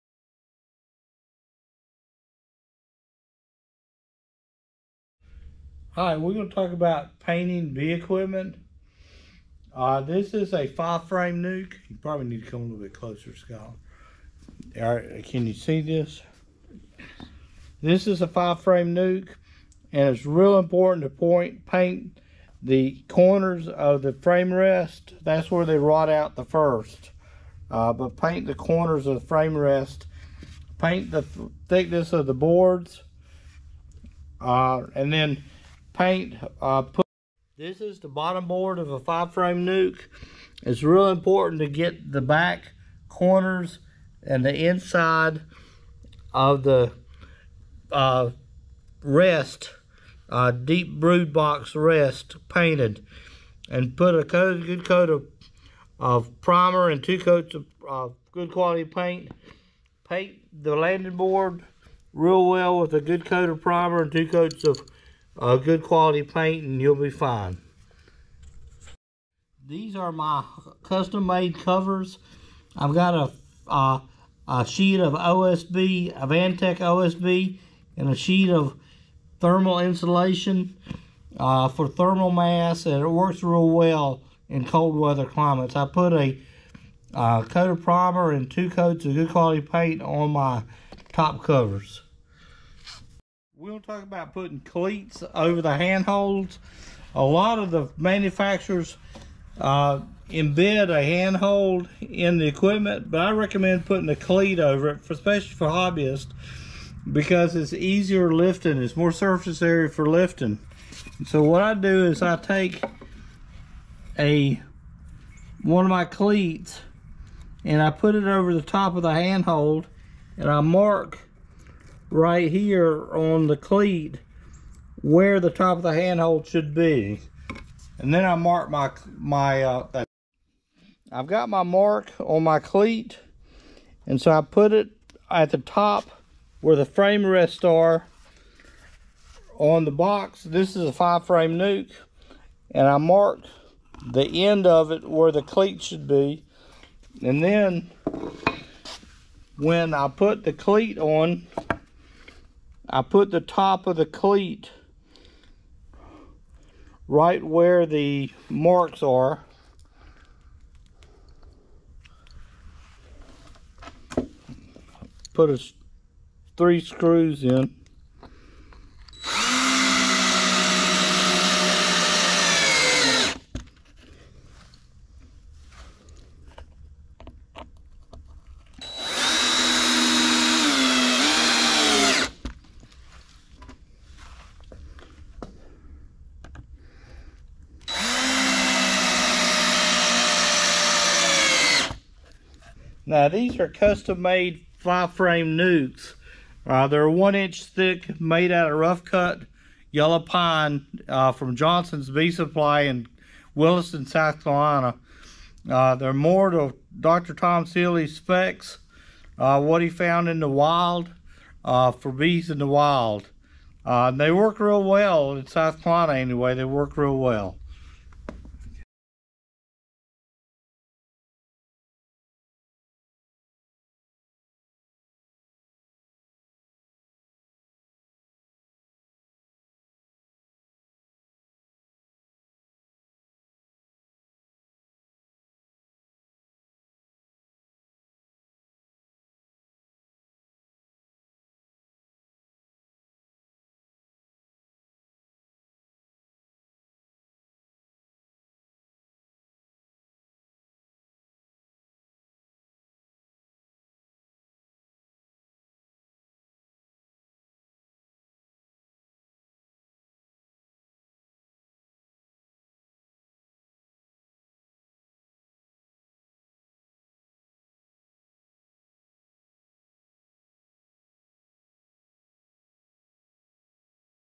painting hives and cleats
Tips for painting bee hives and adding cleats/handholds for ease of lifting